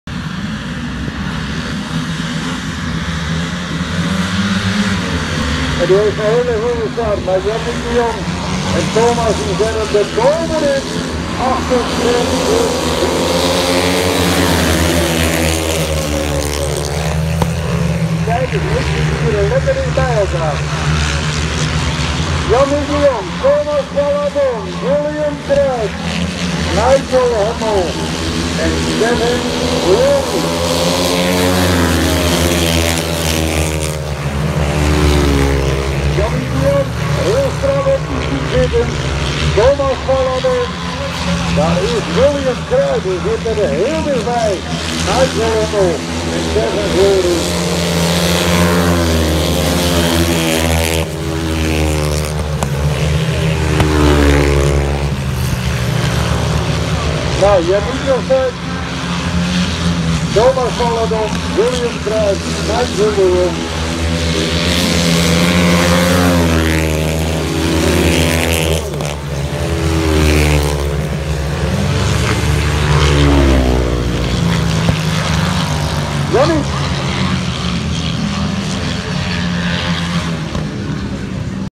Dutch Open Grasstrack Uithuizen 26.08.2022 Solo Lauf 7 Grasbaan Grasbahn
Solo Lauf 7
Kevin Glorie, Jannick de Jong, Nigel Hummel, William Kruit, Thomas Valladon